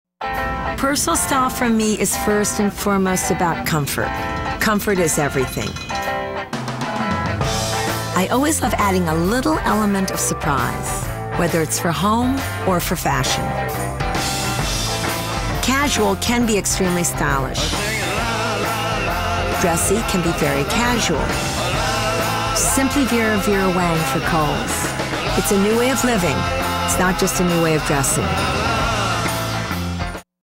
Simply Vera-Vera Wang Kohl's Commercial
Vera Wang herself describes her new clothing line, sold exclusively at Kohl's!